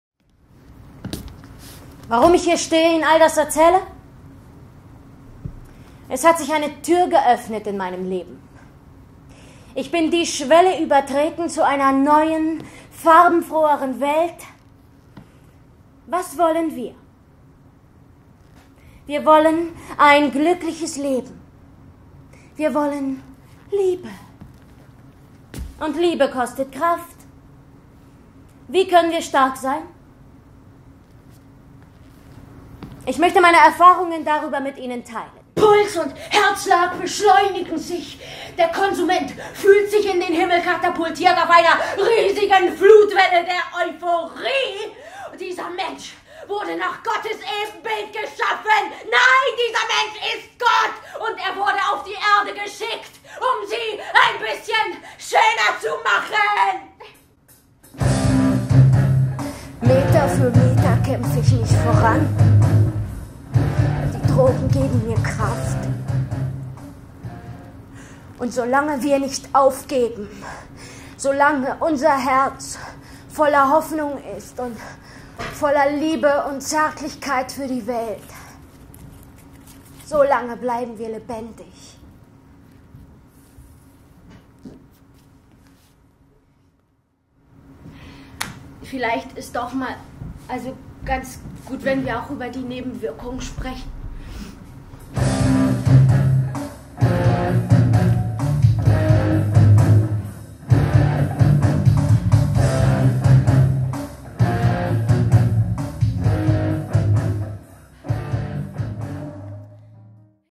Welche Droge passt zu mir?
von Kai Hensel
Spiel: Ines Nieri
Regie: Hans-Peter Kurr

26. - 29. November 2014 // 20:00 Uhr